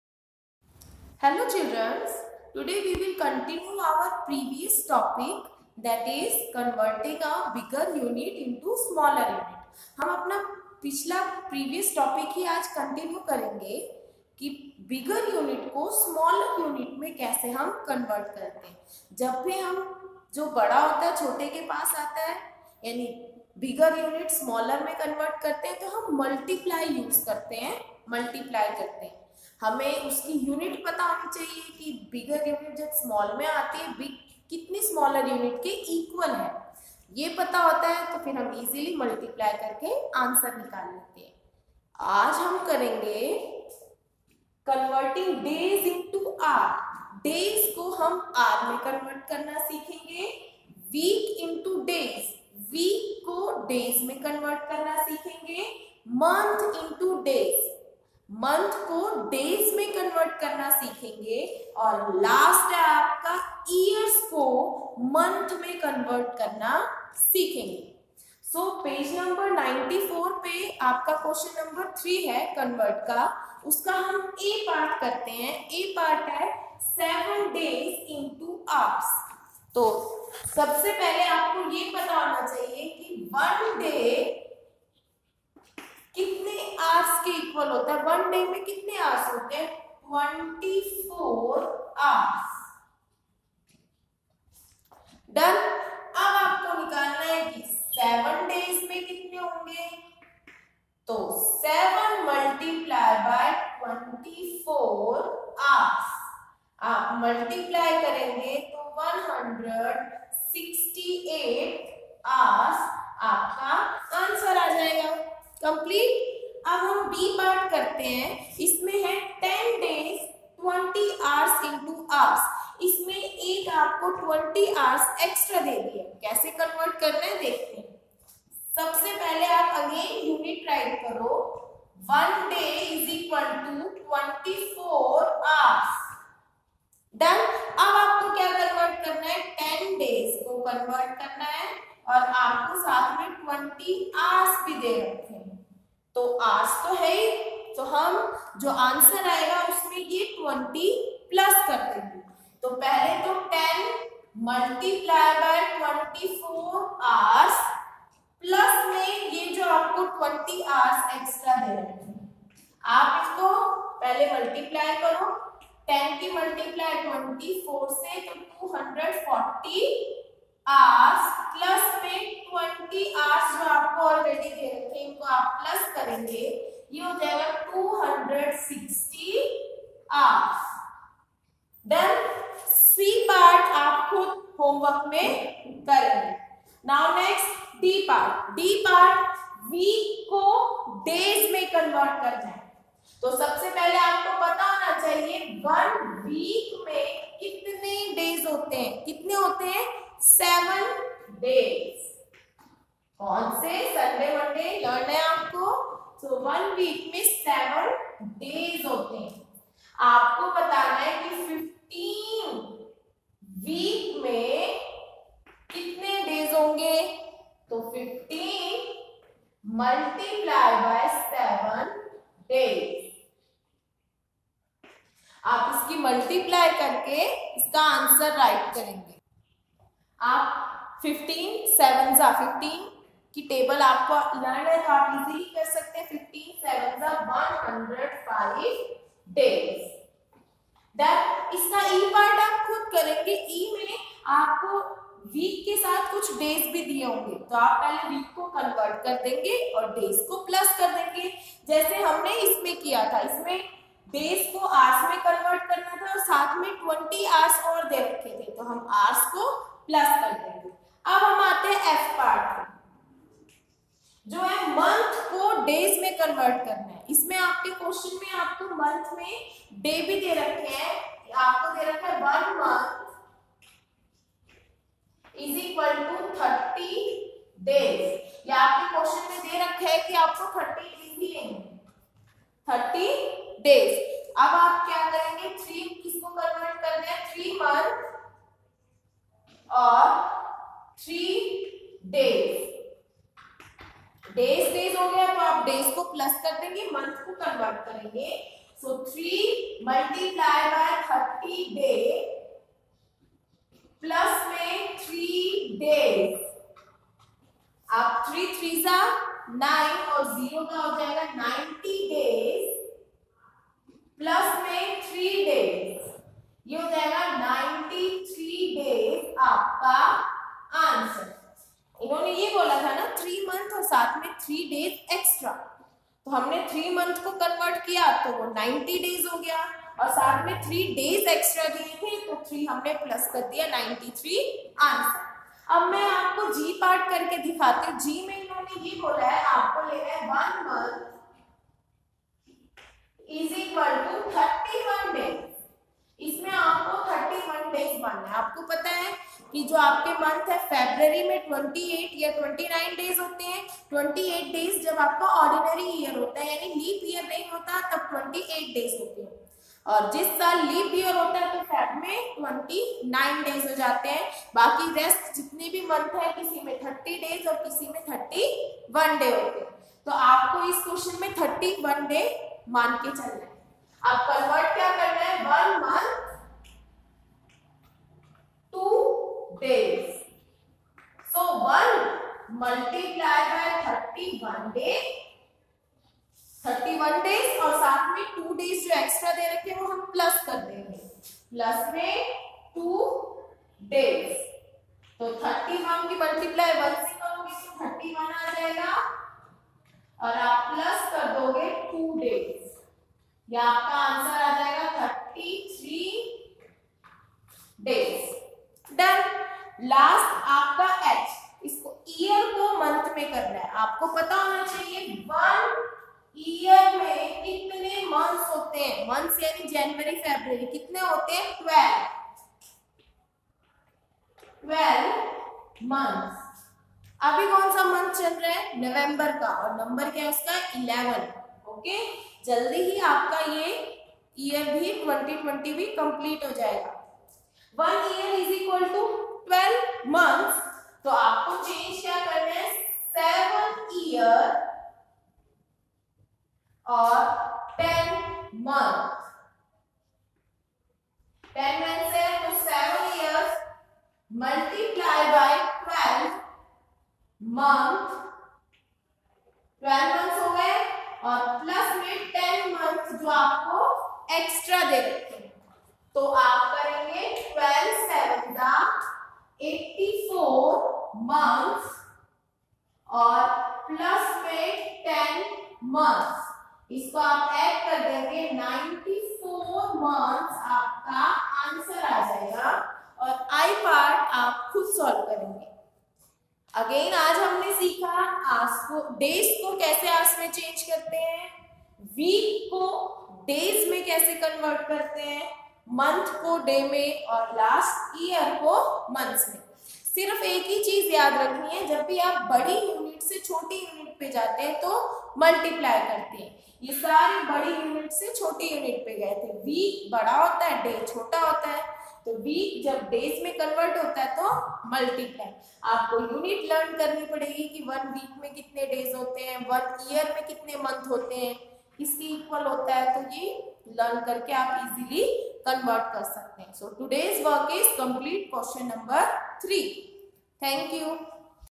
Class - 5 Maths Ch - 8 Times Topic : Converting Days into Hours, Week into Days.
Converting Bigger Units into smaller Units.(part 2)

Days into Hours
Week into Days
Months into Days
Year into Months.